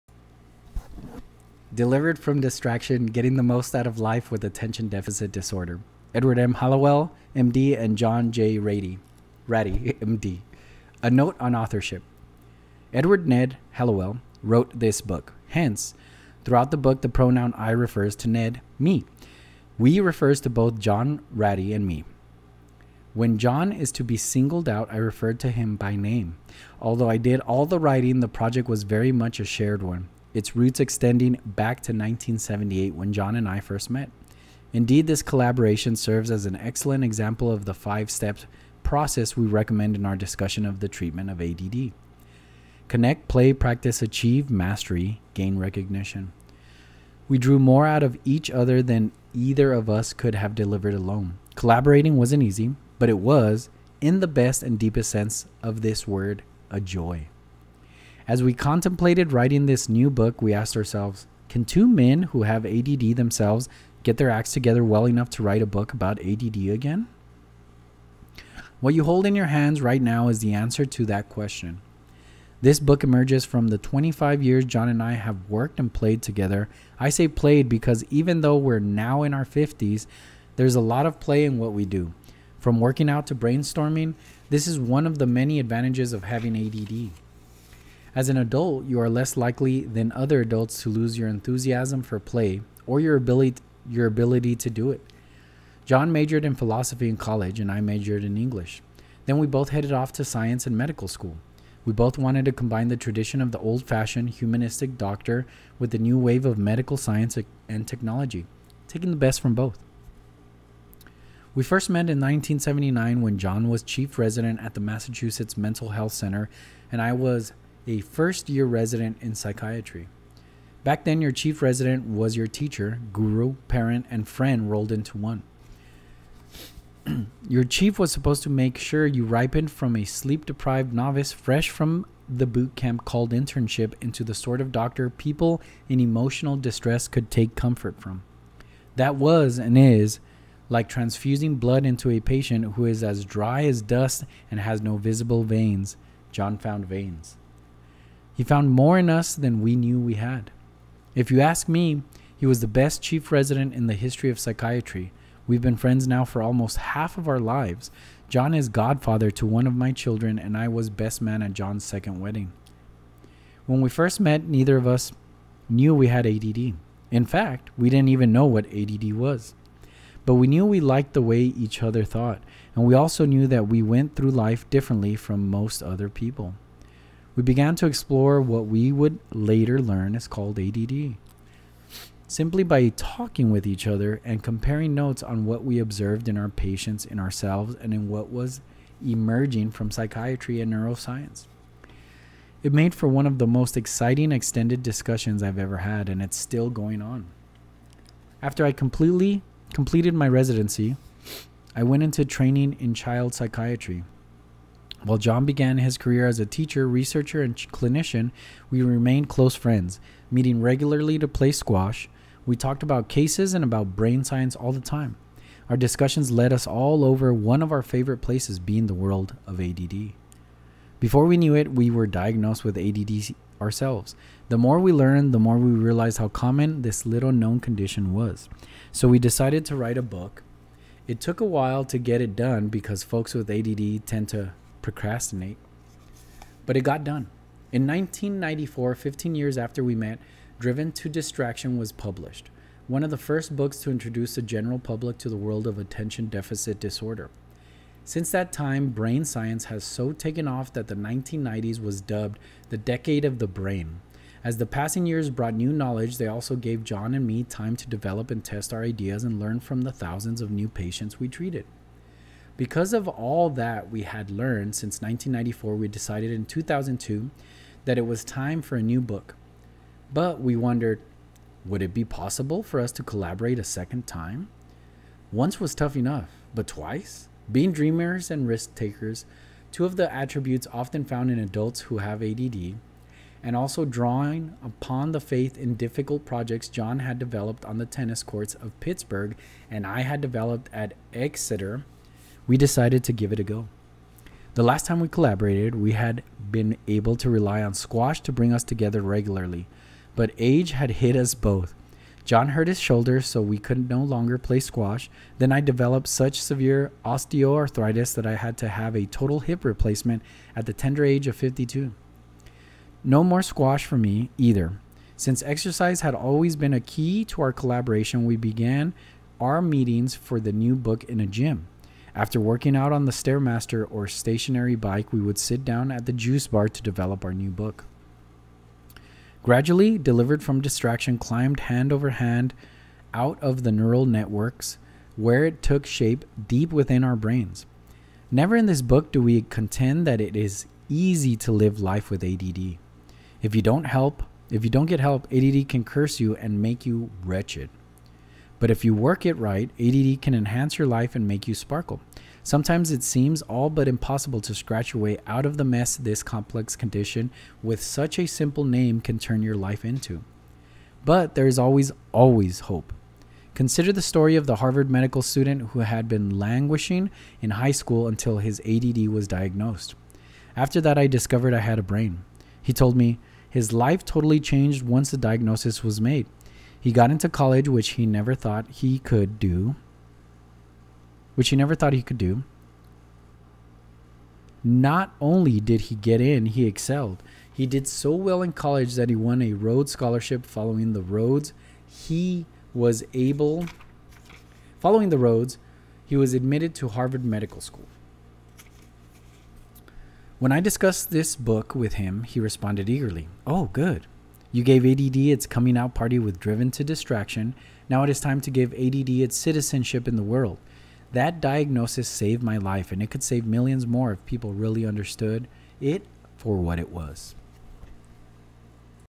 Delivered from Distraction - A Note to Authorship
Audio Reading of
Delivered from Distraction by Edward M. Hallowell, M.D., and John J. Ratey, M.D. - A Note to Authorship

Read Delivered from Distraction
Try Audible for Free